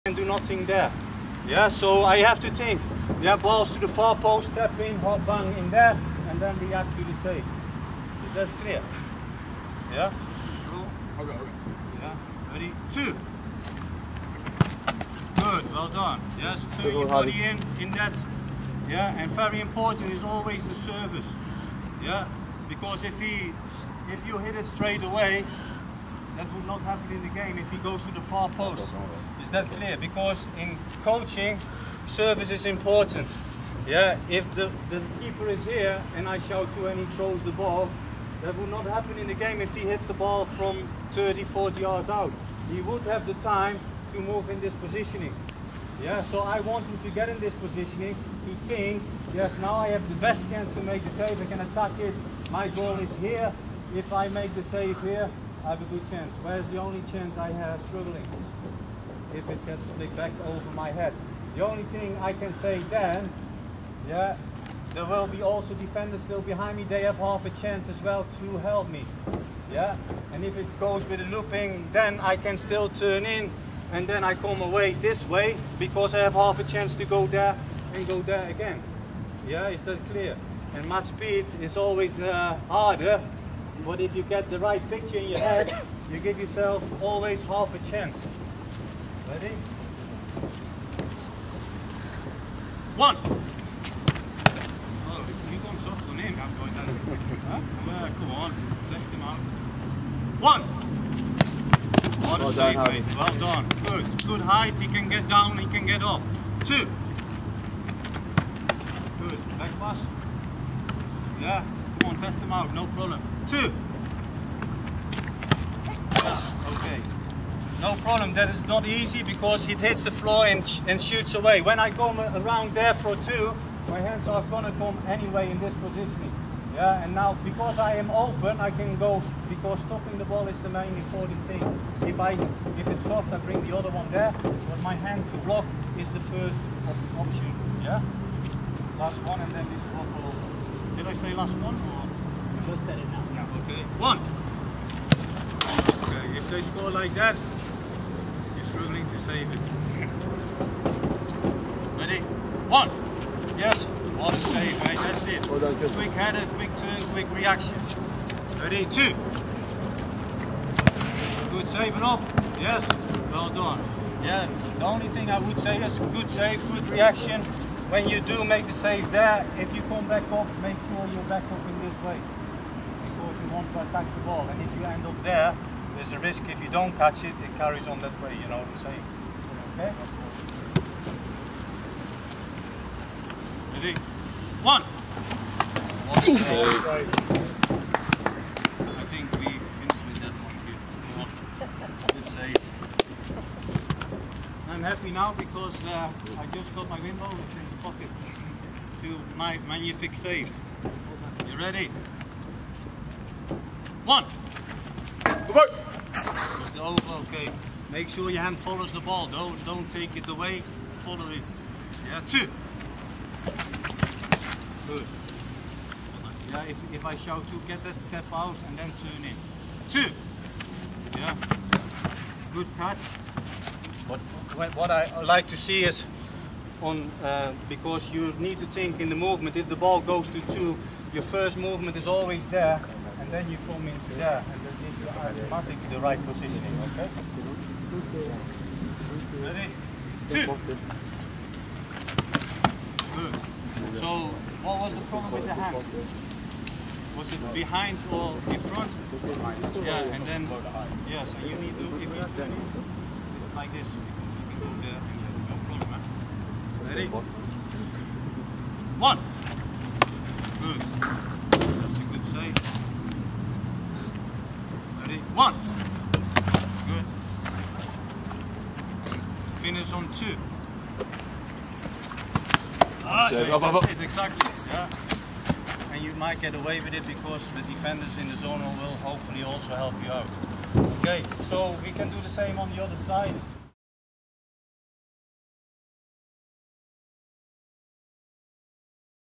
John Achterberg Goalkeeper Training Session
Here's a look back at part of the 2018 IGCC field session with the Liverpool FC 1st Team Goalkeeper Coach, John Achterberg.

We are excited to have John back for the 2022 International Goalkeeper Coaches Conference this June.